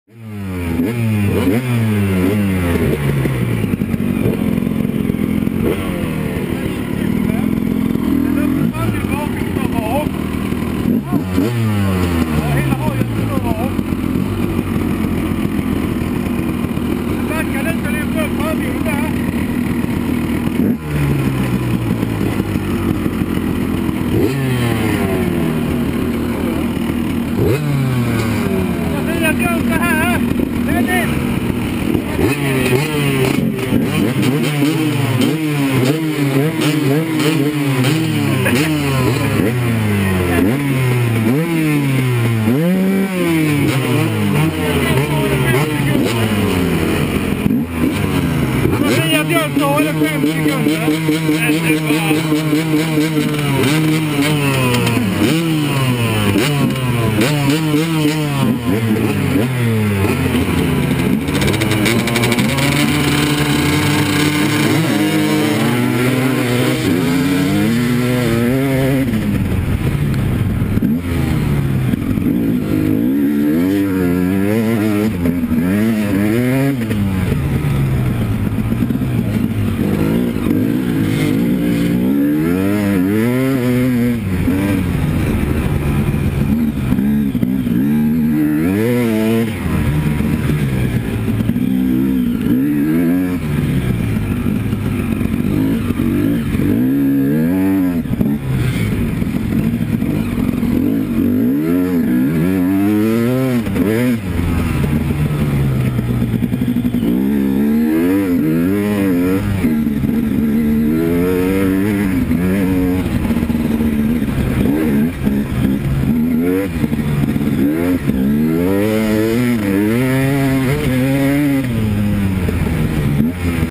KTM 2-stroke riders! 300 EXC 250 SX + EXC
This start was too funny! My mate Dennis imitates the "Most Exited Kid Ever Motorcross" :)